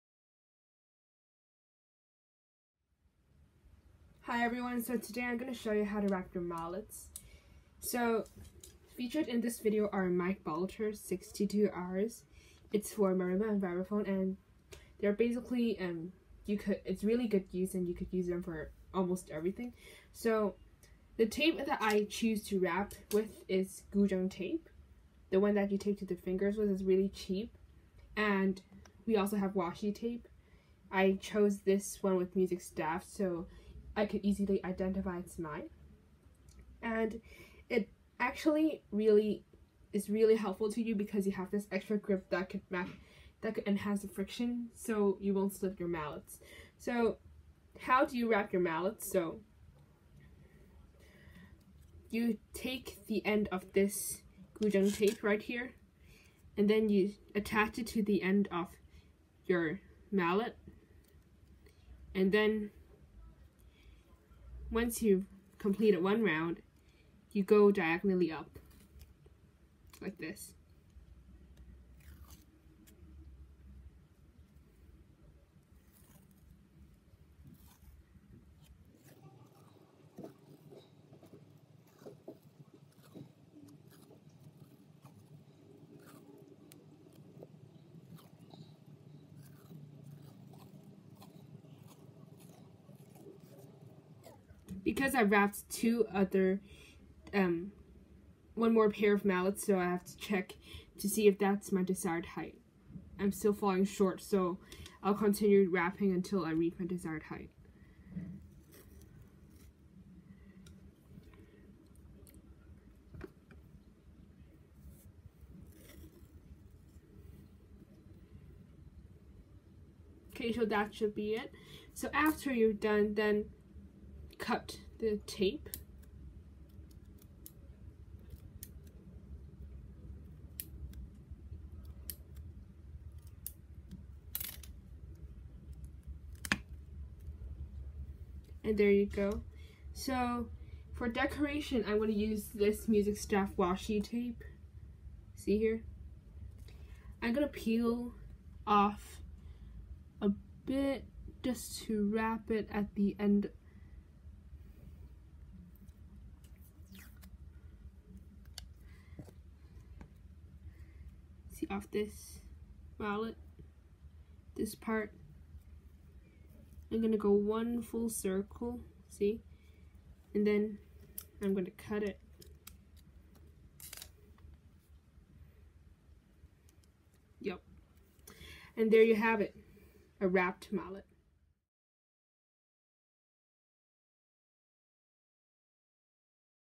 Mallet Wrapping 101
this is a short tutorial video on how to wrap your mallets:) have fun!
credits to my sister sum yin for teaching me because i suck at visual arts:0)
featured mallets: mike balter 62r

my sister’s channel: sum yin ng